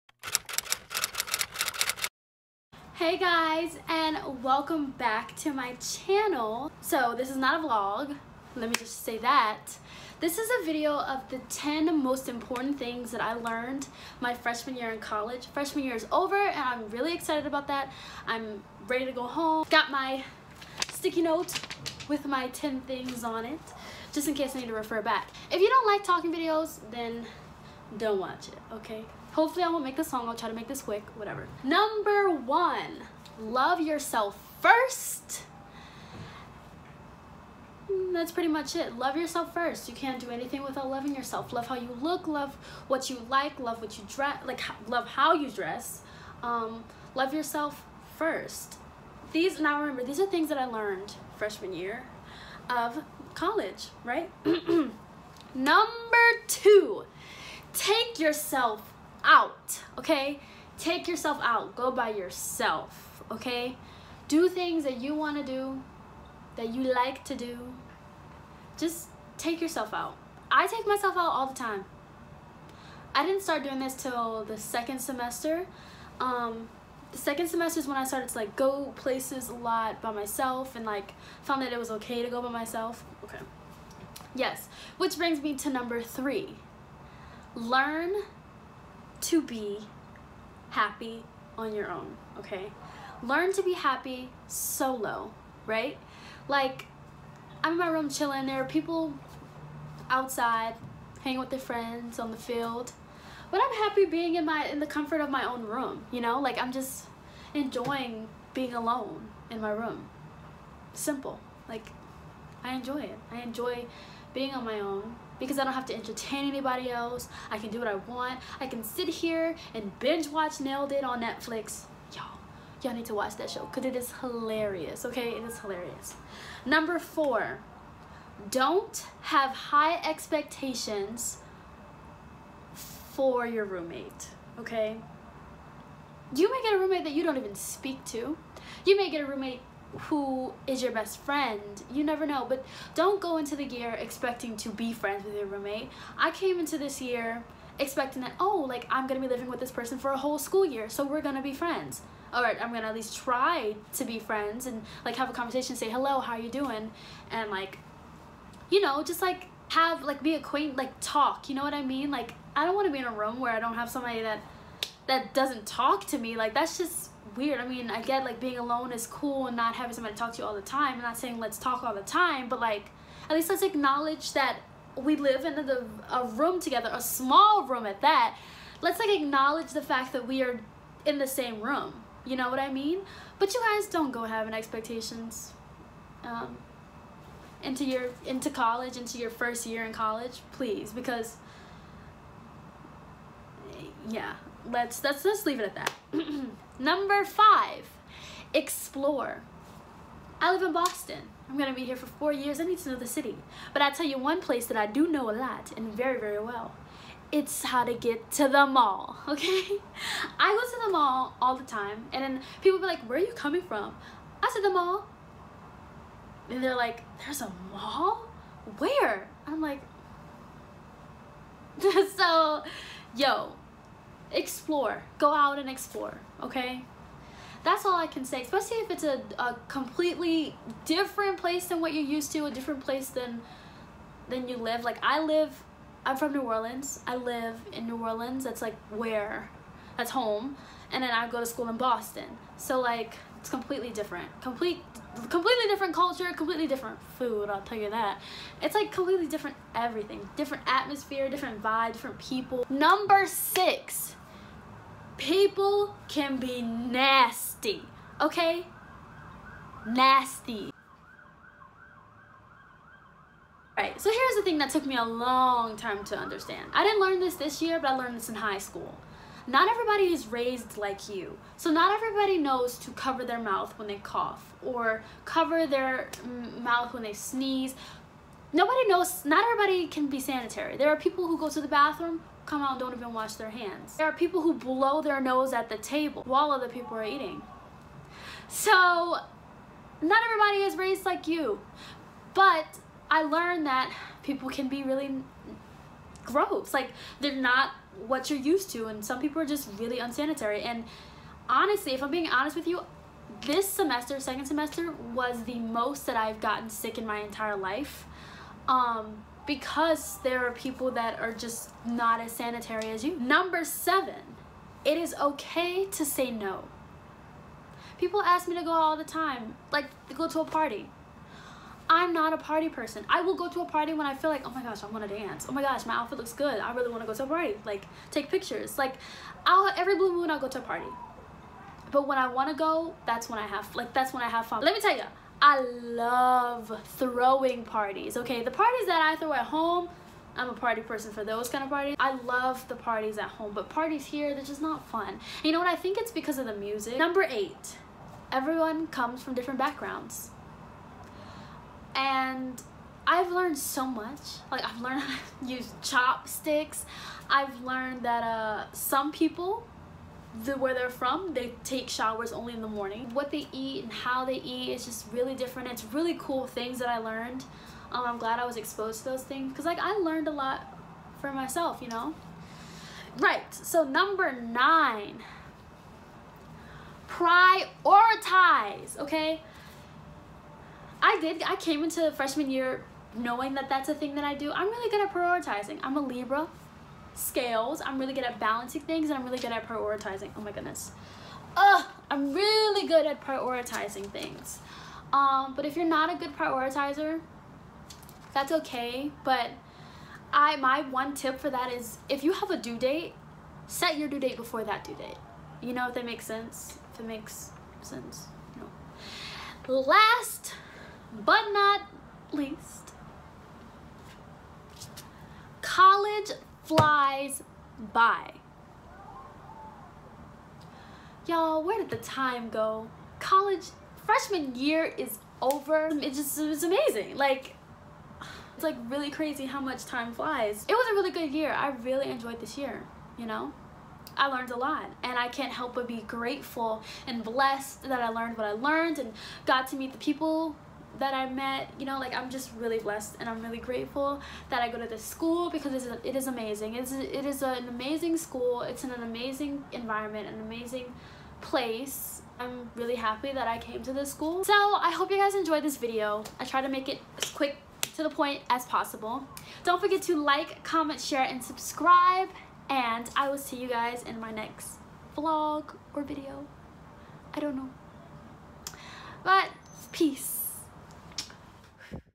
TOP 10 IMPORTANT THINGS I LEARNED FRESHMAN YEAR OF COLLEGE
Here are the top 10 most important things I learned my first year of college! I hope you can enjoy, learn, or relate!